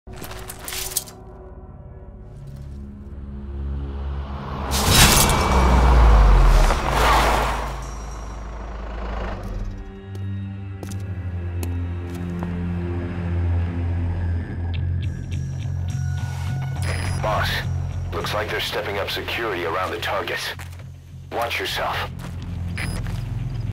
(SFM) MGS V: SFM Cinematics Test
I wanted to do a quick test of MGS:V cinematography style. It's kind of a proof of concept and practice of scene creation of some sort. Fun fact: I repurposed old motion capture I did for previous films such as Protector and HL3 Fan Trailer.